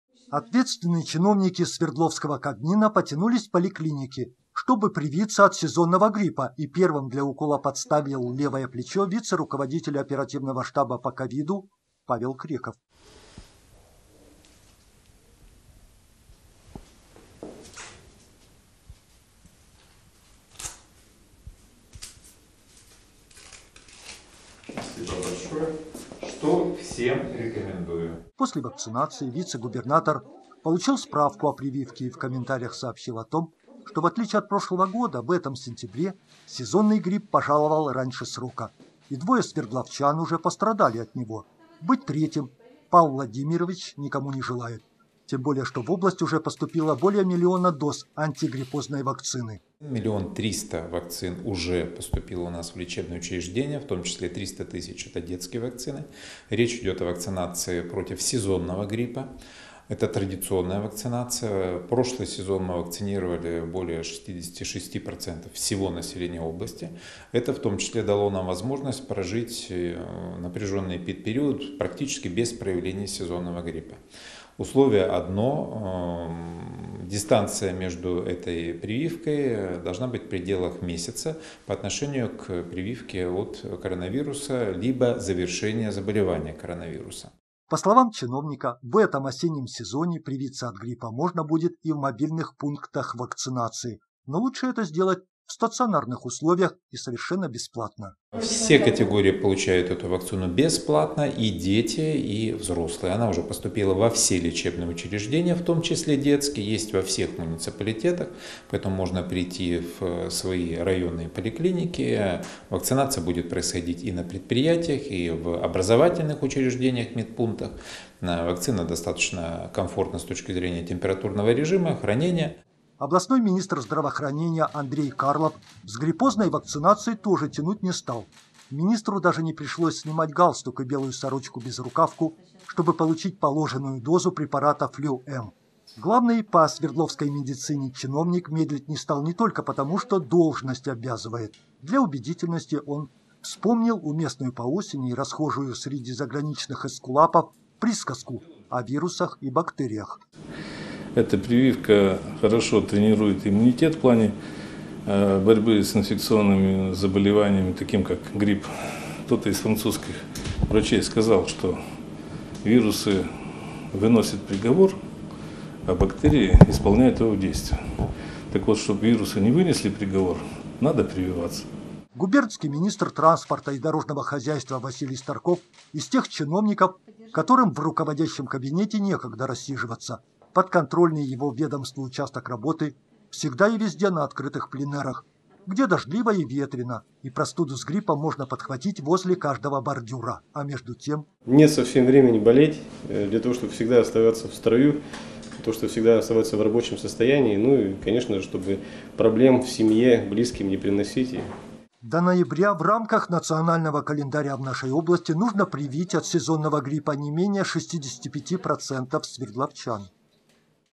Чиновники первыми укололись от гриппа
Первым подставил плечо вице-губернатор, ответственный в кабмине за COVID, министры тоже не стали тянуть с прививками от сезонного гриппа